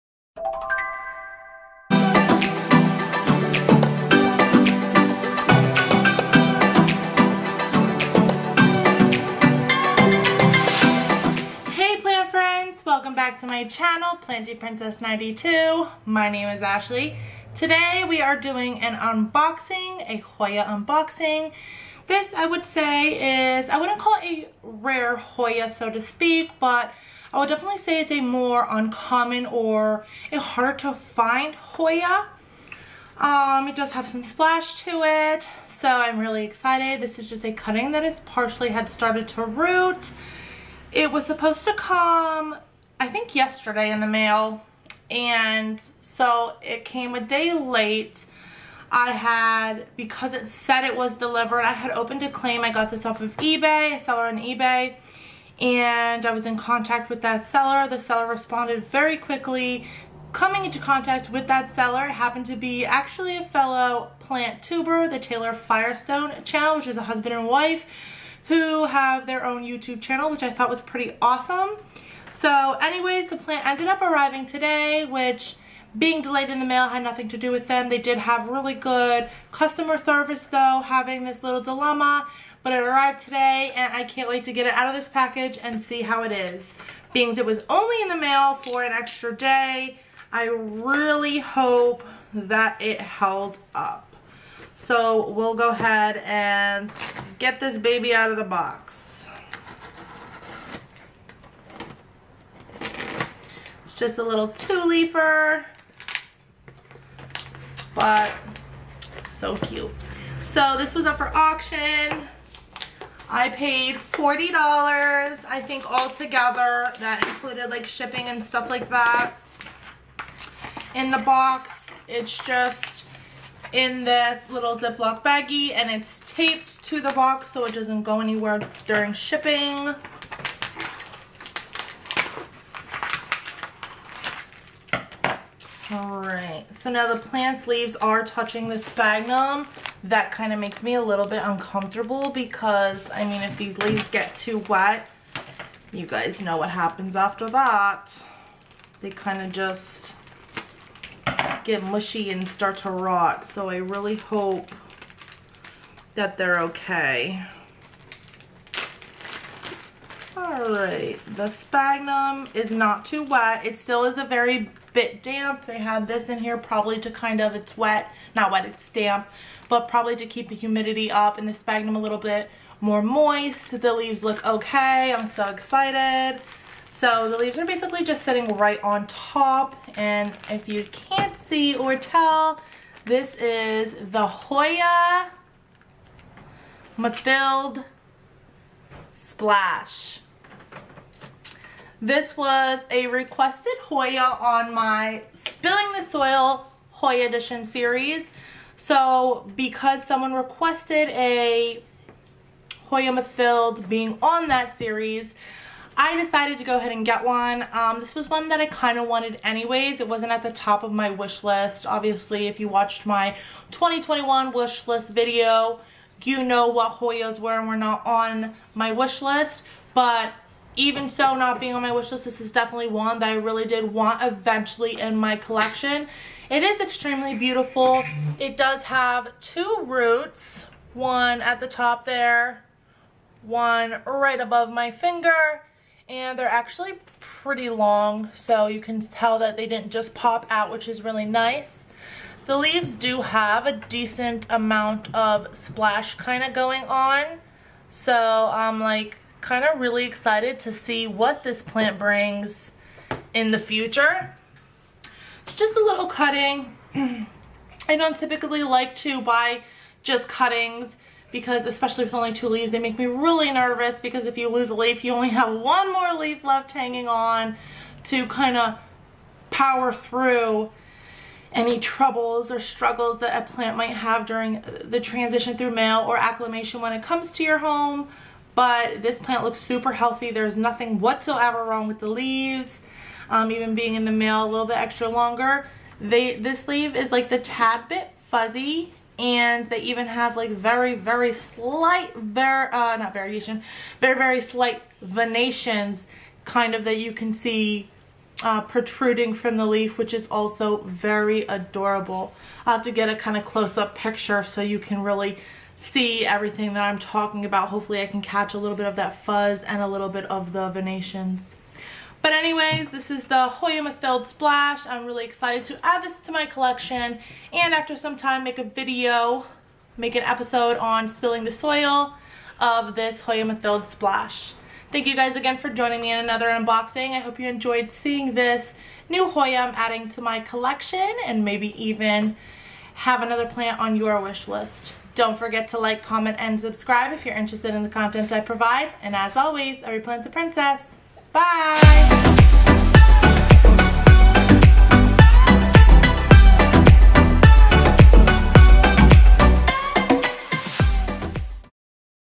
UnBox with ME! ||Hard to find Hoya|| Hoya Mathilde Splash!
Happy Easter to my beautiful plant peeps who celebrate this holiday! Happy Sunday to beautiful plant pals who don’t! Today I’m unboxing a hard to find Hoya that was requested on my “Spilling the Soil!” series! The HoyaMathildeSplash! This has been a Hoya that wasn’t at the top of my wishlist but definitely one that I’ve been admiring and wanted to add to my collection for some time so I’m so excited to finally add this cutie patutie to my Hoya Collection! I hope you enjoy seeing this plant as much as I do and are inspired to go beyond your comfort zone and open up to everything green!

Don’t forget to check out my Plantagram @ for daily plant content!